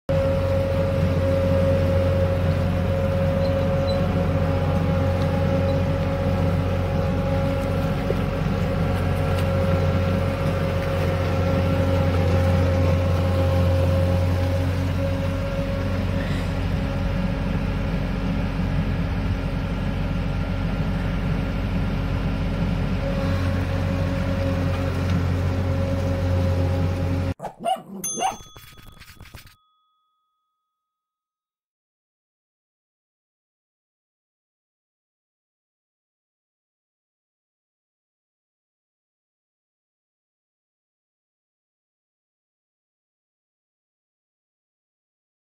Cattle dog joins owner for a day of work in a skidsteer
Cooper loves to ride (on and in anything) and work and is owner on the skidsteer is the perfect opportunity to do both. Cooper will run and jump on it if his owner even gets near the skidsteer. He found a new passion. He is so funny!